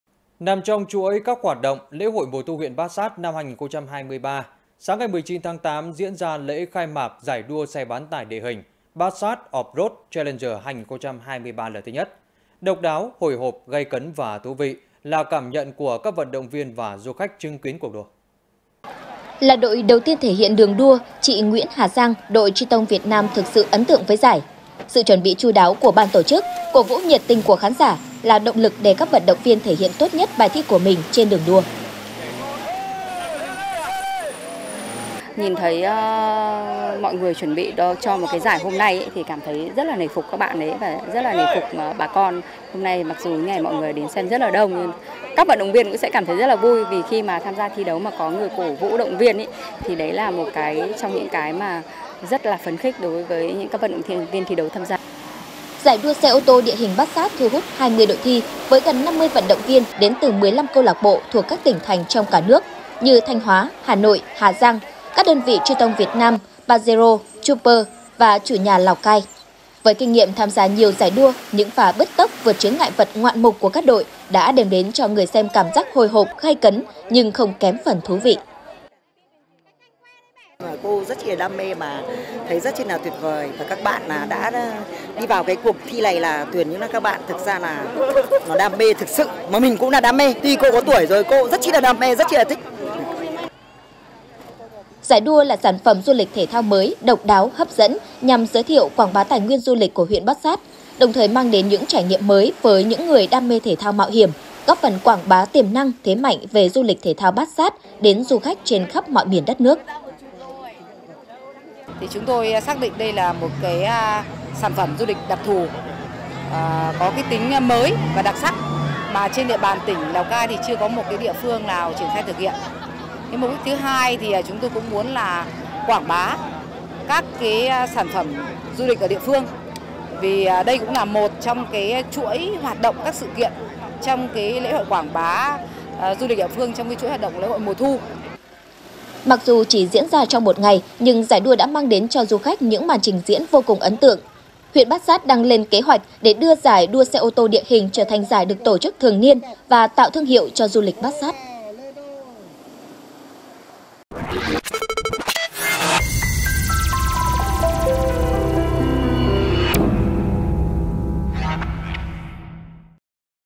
Offroad - đam mê bất tận | THLC
Laocaitv.vn - Nằm trong chuỗi các hoạt động “Lễ hội mùa Thu huyện Bát Xát năm 2023”, sáng 19/8, diễn ra lễ khai mạc Giải Đua xe bán tải địa hình “Bat Xat Offroad Challenger 2023” lần thứ Nhất. Độc đáo, hồi hộp, gay cấn và thú vị là cảm nhận của các vận động viên và du khách chứng kiến cuộc đua.
Đài Phát thanh - Truyền hình Lào Cai